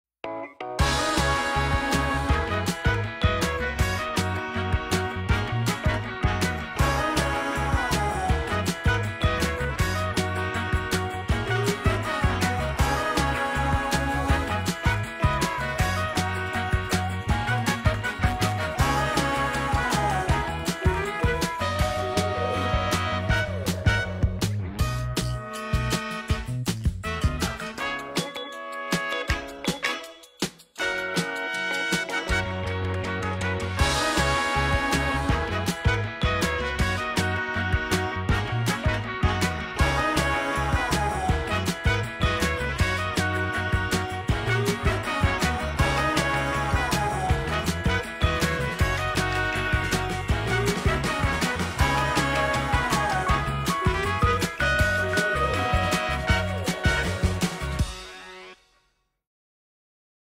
This is Changi...
I Took a trip to Singapore, but on the way i had to go see the airport!! Here's a sneek peek!!